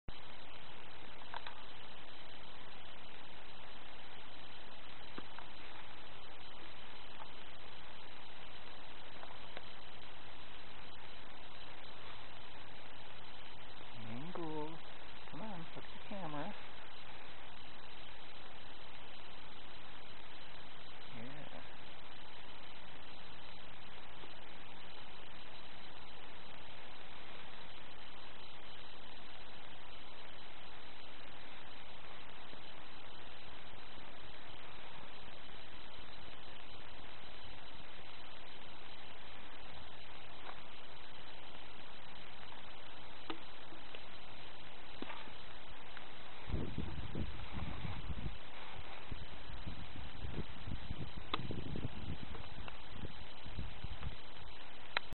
eBay Item Demo - HP PhotoSmart M517 Camera
Demonstration of the video recording capability of an HP PhotoSmart M517 5MP camera going up for sale on eBay.  Although a decent camera overall, the video quality is limited to 320x240, good but not great.

Cat not included.